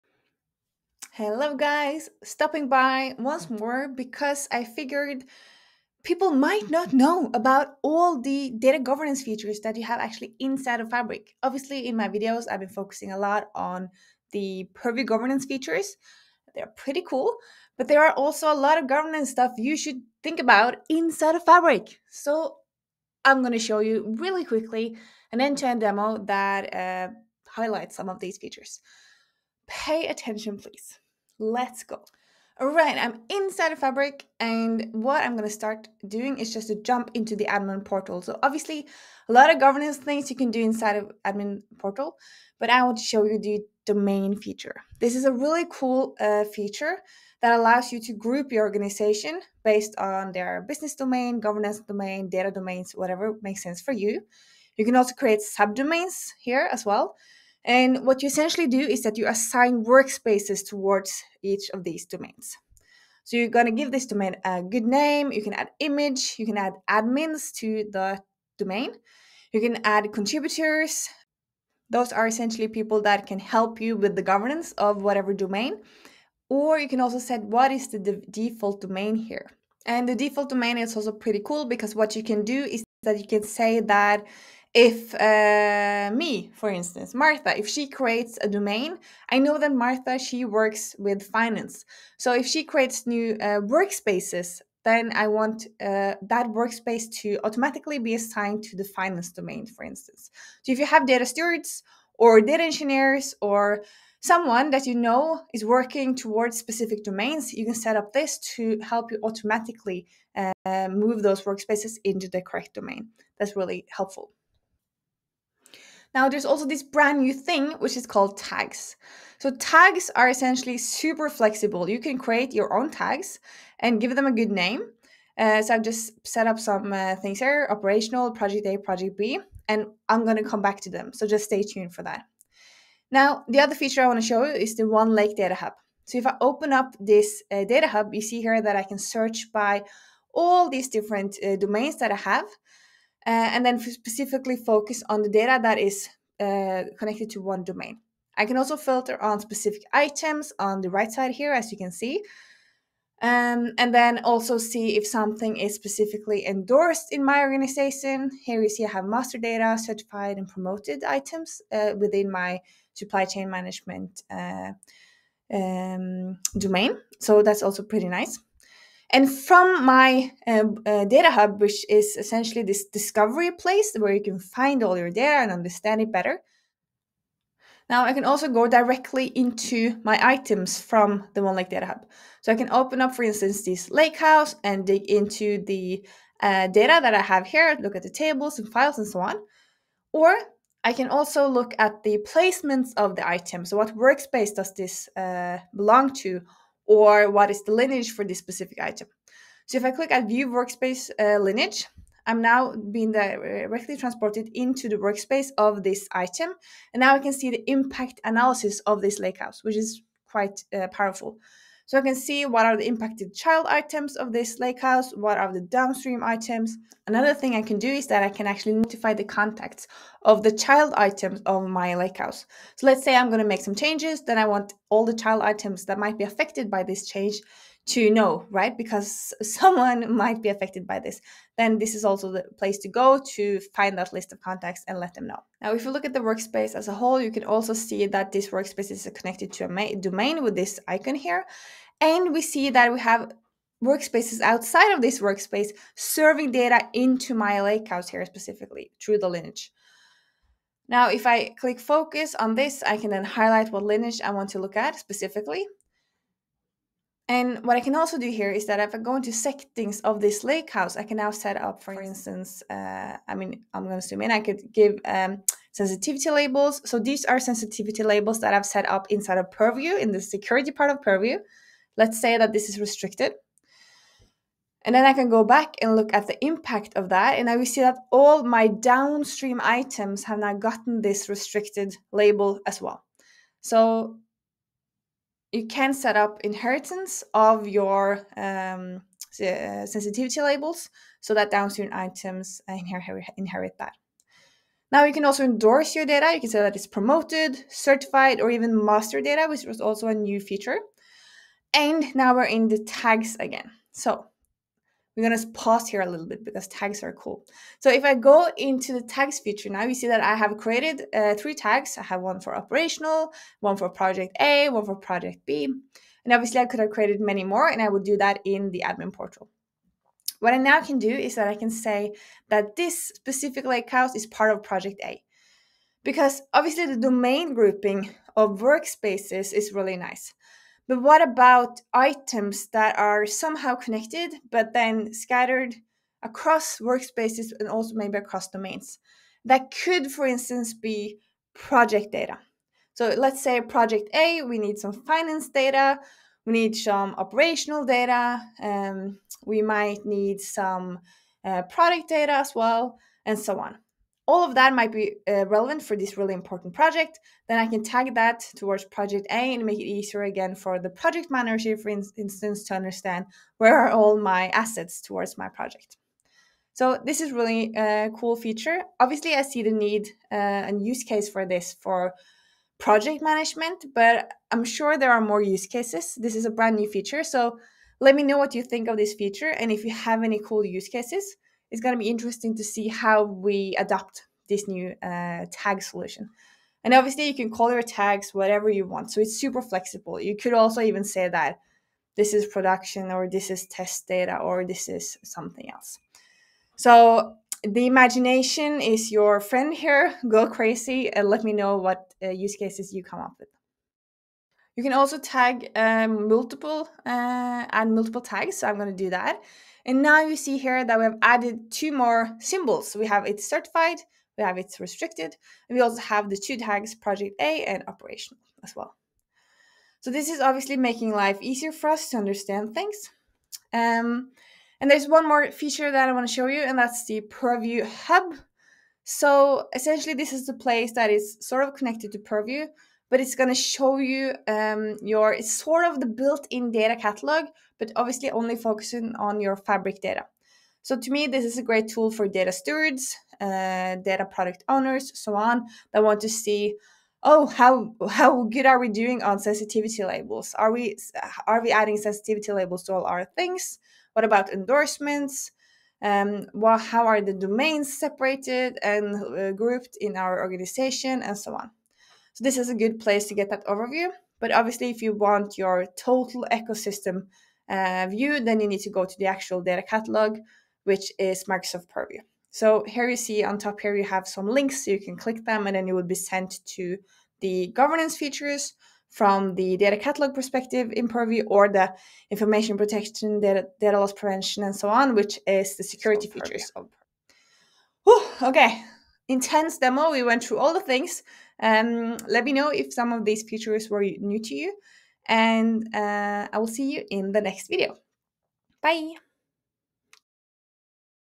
All the Governance Features in Microsoft Fabric - an end-to-end demo!
I've put together an end-to-end demo exploring all the essential governance features in Microsoft Fabric! In this video, I dive into:

✅ Domains
✨ The brand new Tags Feature
🔗 Lineage
🌐 OneLake Hub
🌟 Endorsements
🔒 Sensitivity Labels
🔍 Purview Hub

These tools are game-changers for organizing, tracking, and securing data across your organization, from improving discoverability to strengthening compliance.

Check out the video to see how these features can support your data governance goals!